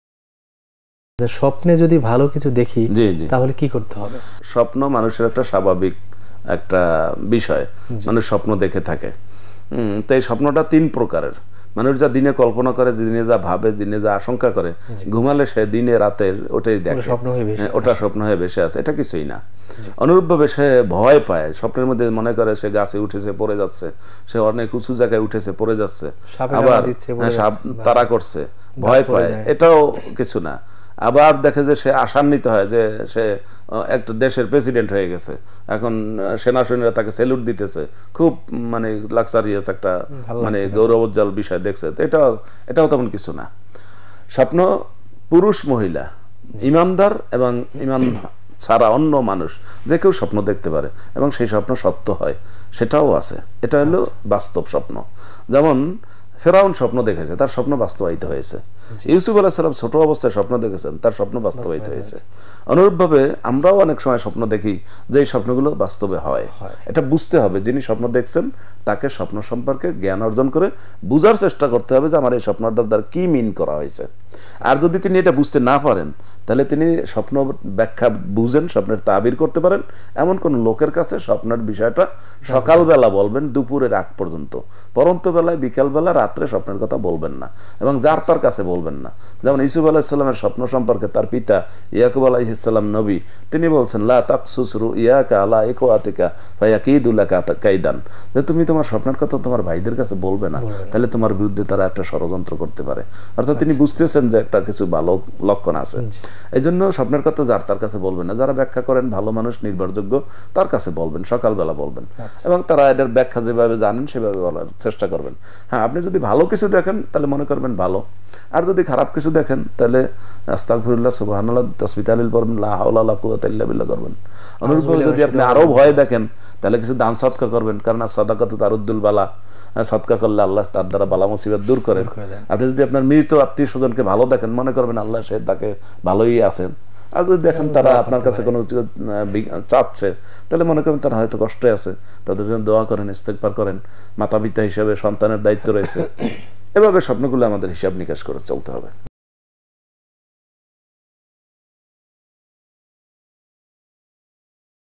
স্বপ্নের ব্যাখ্যা | শাইখ গাজী মোহাম্মদ আবুল আবুল কাশেম | ইসলামী প্রশ্ন ও উত্তর
প্রশ্ন: স্বপ্নের ব্যাখ্যা কি?
আলোচক: শাইখ গাজী মোহাম্মদ আবুল আবুল কাশেম
Cheif Advisor: Sharif Bayzid Mahmud
Head Of Program: Mahbub Mukul